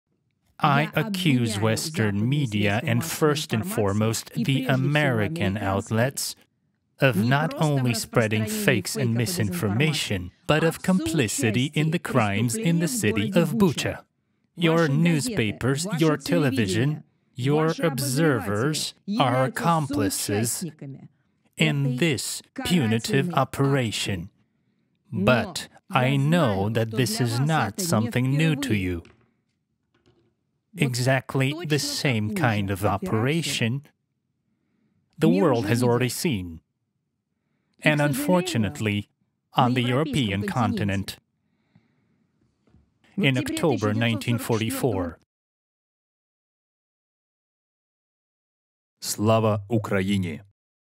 Russian propagandist Zakharova tries to shift the blame for Bucha massacre to the West. English dub.
SAVE LIVES! ASK YOUR GOVERNMENT TO GIVE UKRAINE AIR DEFENCE SYSTEMS. SUPPORT THE UKRAINIAN ARMY